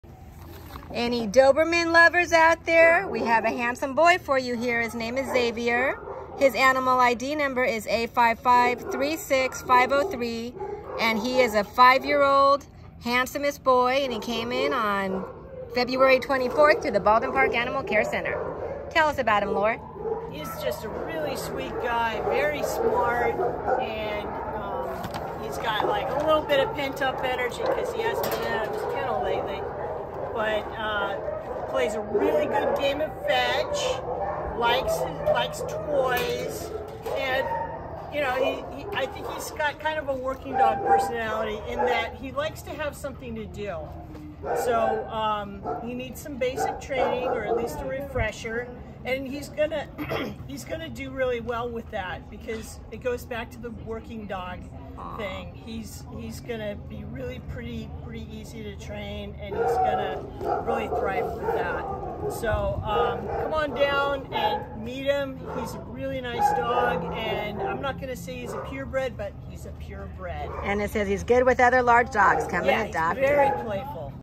NEW! A5536503 Xavier | Doberman Pincsher
A5536503 Xavier is a graceful 5 year old red and brown neutered male Doberman Pinscher who was found as a stray and brought to the Baldwin Park Animal Care Center on February 24. Weighing 57 lbs Xavier is tall and lean and moves like a gazelle. He is a handsome red Doberman who also has a goofy playful side. He likes to play fetch and chase toys. Curious and smart, Xavier already knows sit and shows signs of housebreaking. He just needs some training and positive reinforcement to walk politely on leash. For now the shelter is requiring that any children in the home are at least 8 years old to be the best playmates for him, and he’s fine to go to a home with other large dogs. Xavier is an all around great dog who will bring a lot of joy to his lucky adopter.